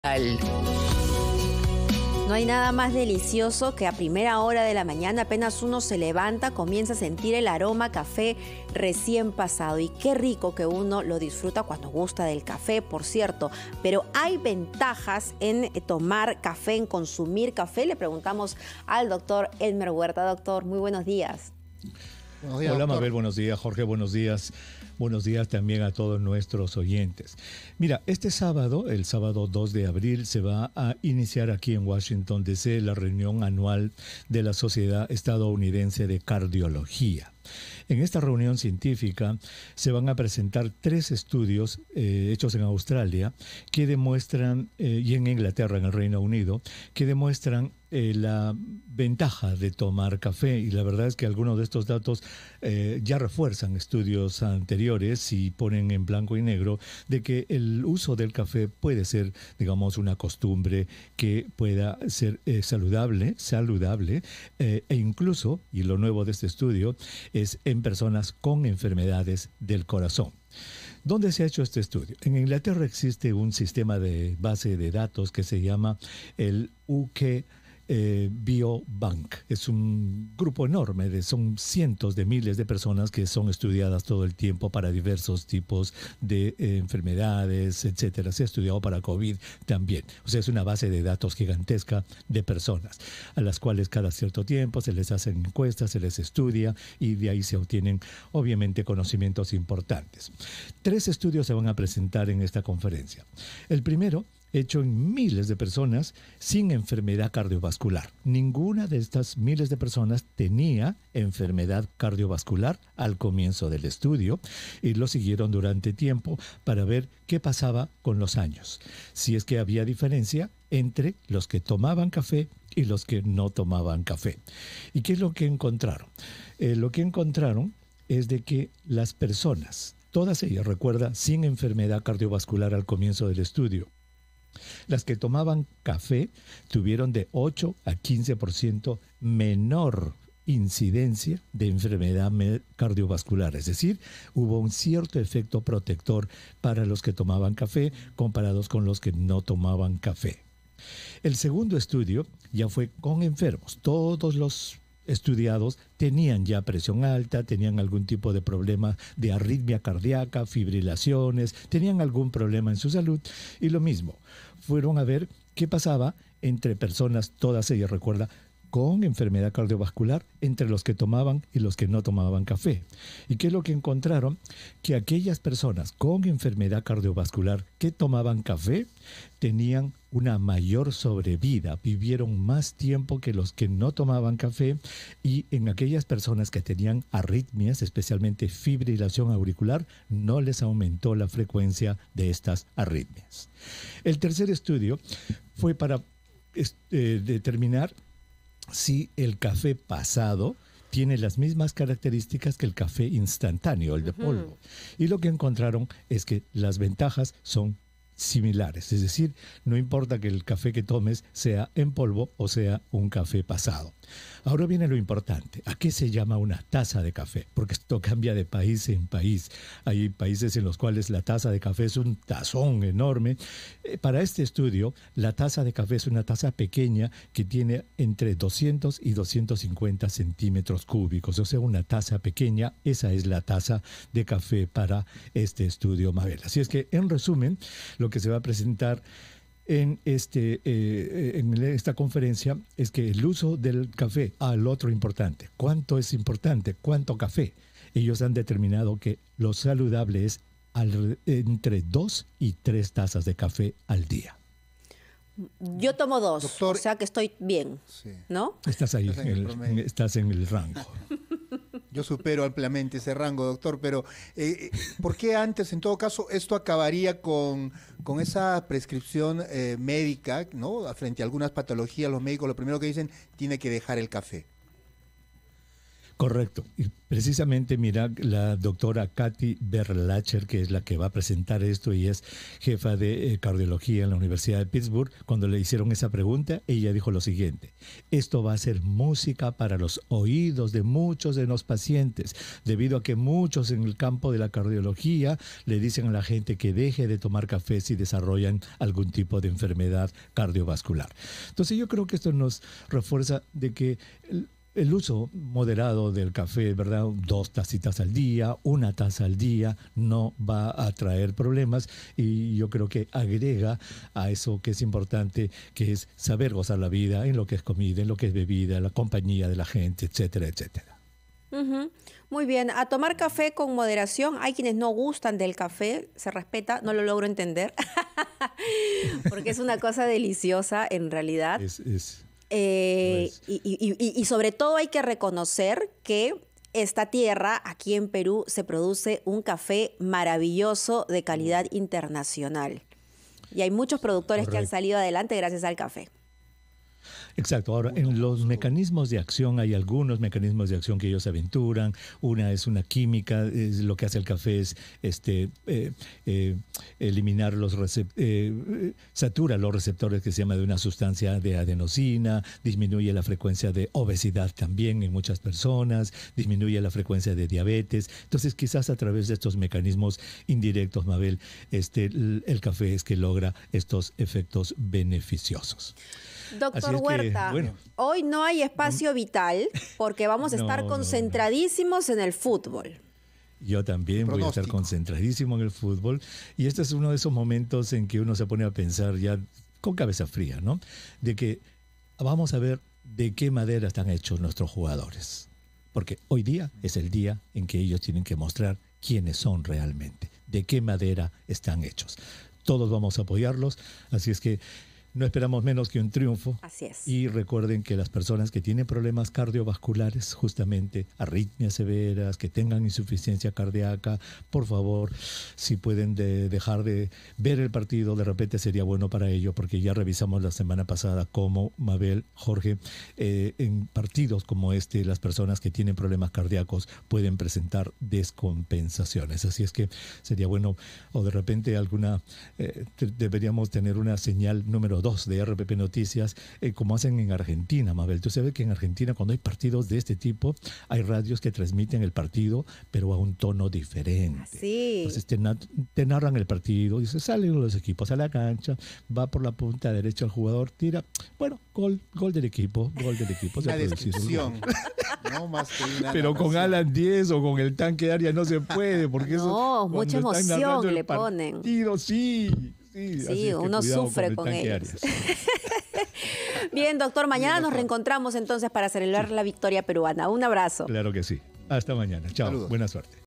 Descubre las ventajas de tomar café para tu salud
El doctor Elmer Huerta comenta cuáles son los beneficios que se han encontrado en estudios de consumo de café.